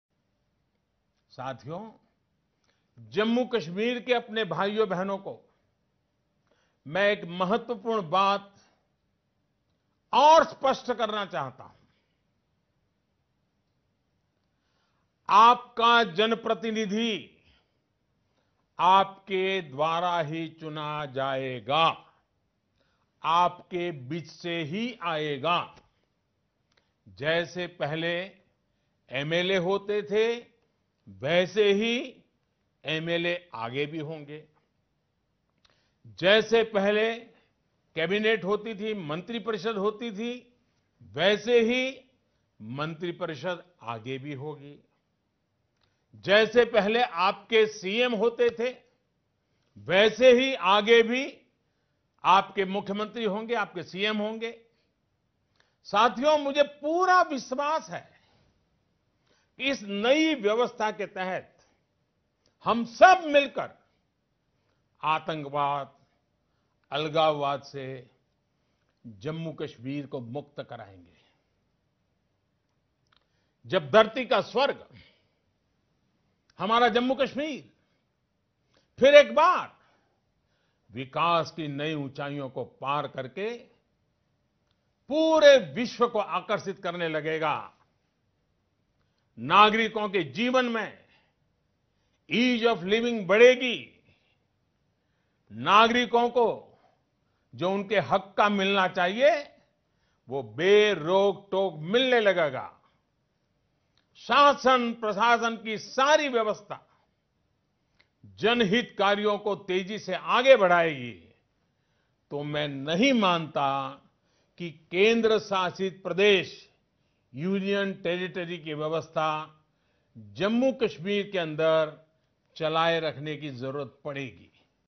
जम्मू-कश्मीर फिर एक बार विकास की नई ऊंचाइयों को पार करके पूरे विश्व को आकर्षित करेगा: प्रधानमंत्री
Prime Minister Narendra Modi's address to the Nation | 8 August 2019  | Article 370